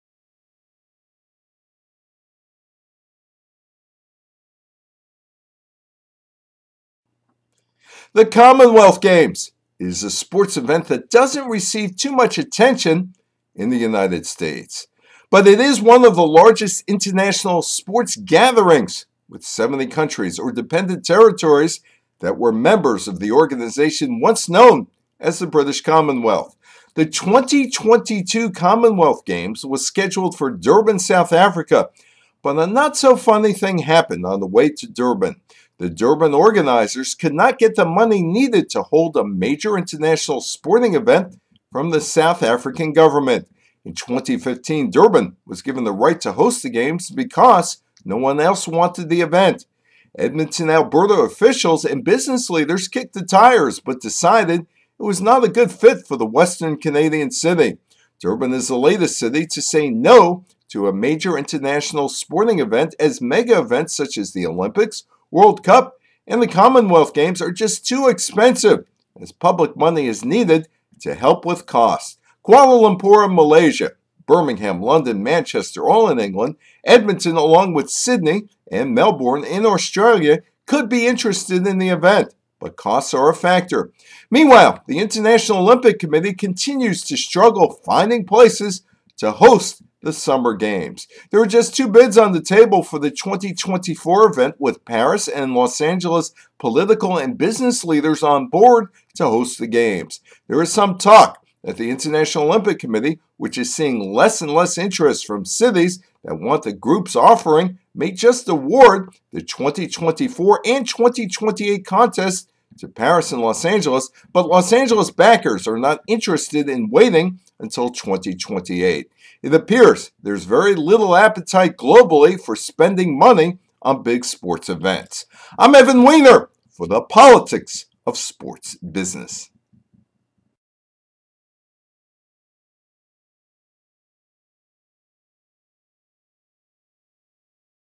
Durban Latest City To Say No To A Major Sports Event
South Africa hosted soccer’s World Cup in 2010 but will not host the 2022 Commonwealth Games.